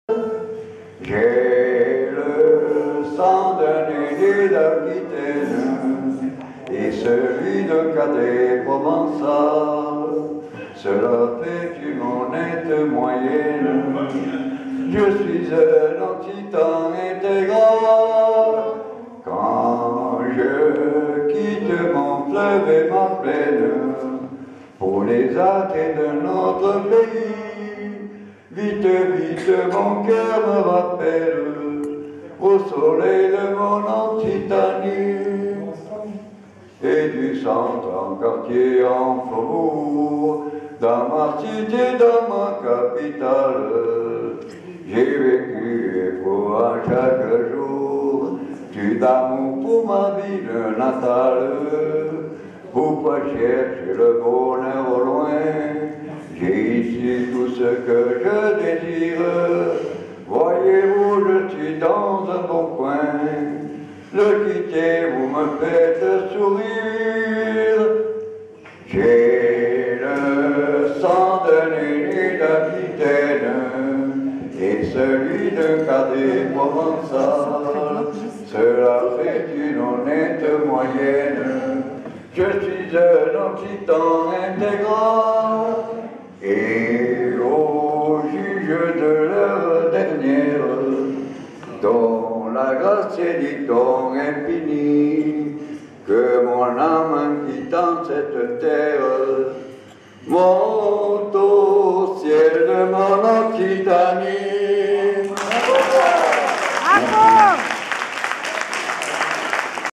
8 mai 2012 ancien combattants 001.MOV
Les anciens combattants à St Pons-de-Mauchiens l'après-midi du 8 mai après le repas. Les anciens font leur show Raymond chante